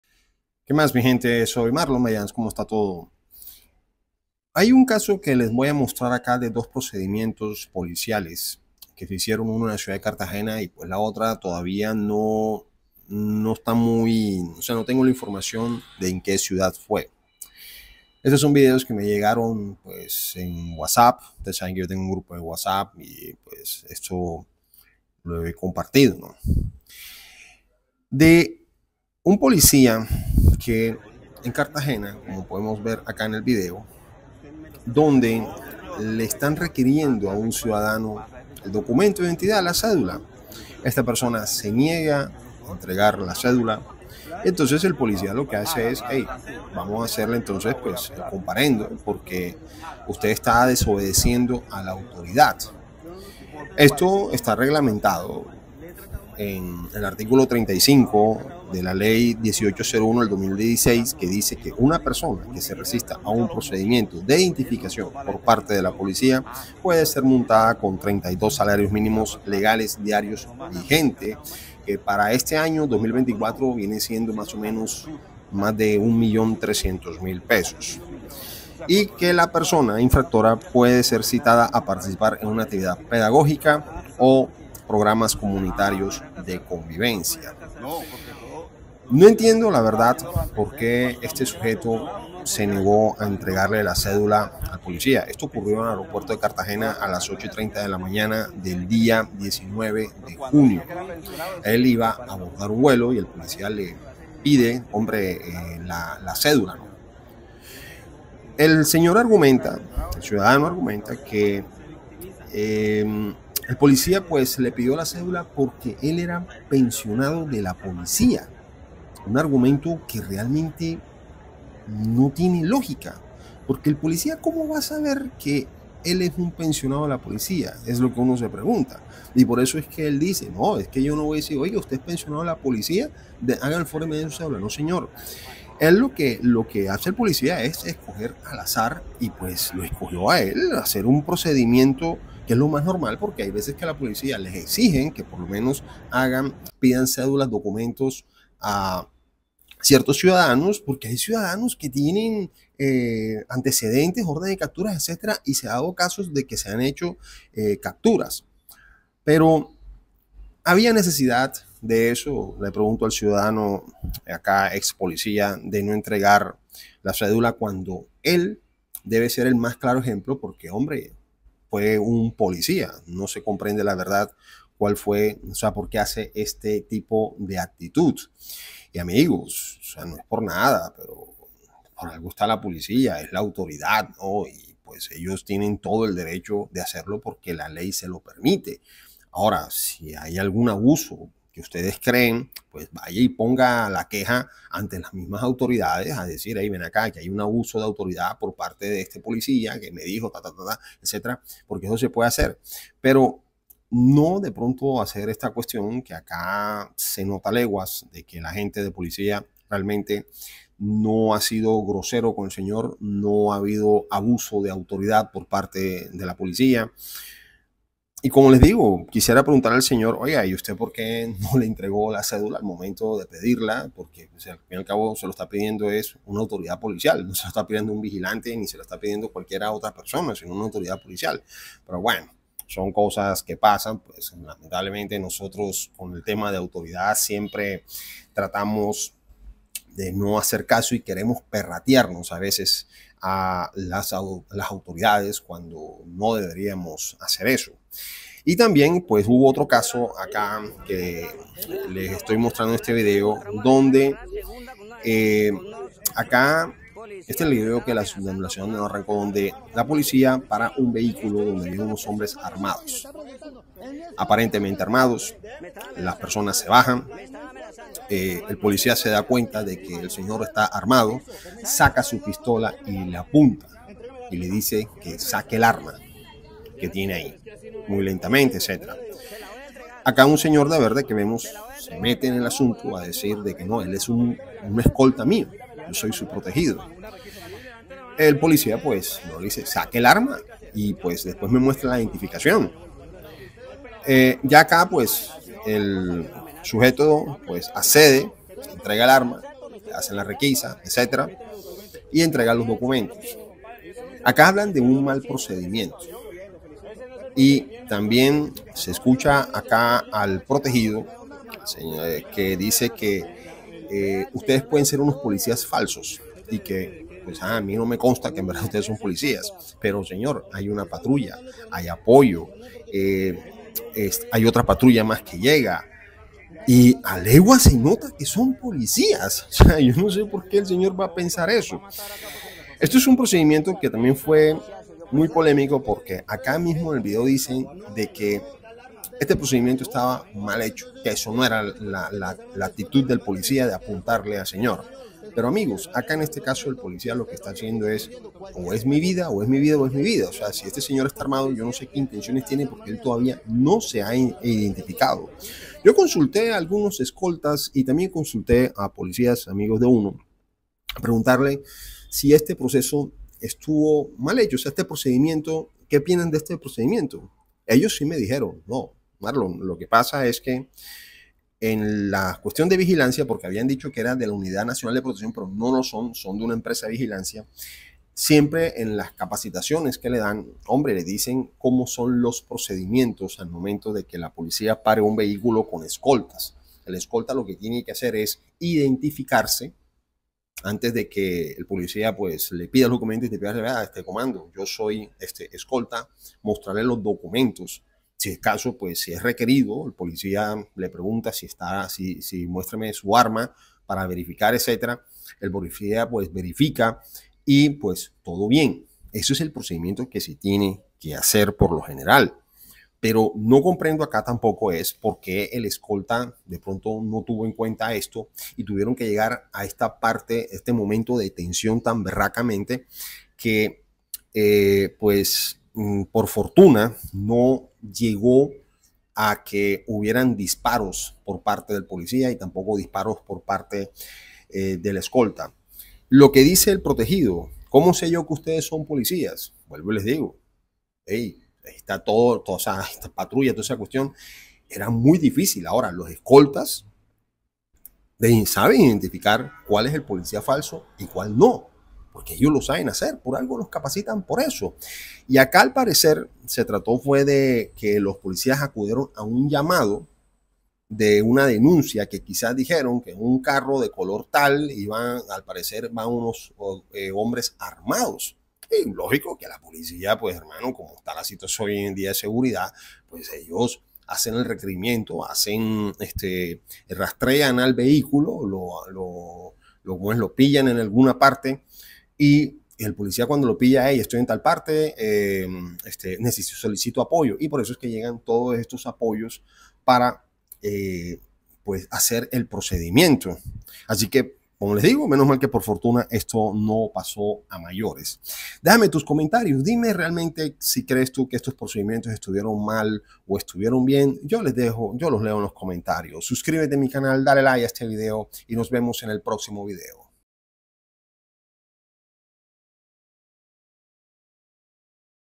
Procedimientos de policías polémicos en redes sociales
En Colombia muchas veces pasan que hay procedimientos mal realizados. Algunos casos que la gente ignora leyes en donde los policías están en todo su derecho de hacer algún procedimiento. en este video te mostraré dos procedimientos que ocurrieron en dos distintas ciudades en donde se discuten si fue malo o bueno estos requerimientos por parte de la policía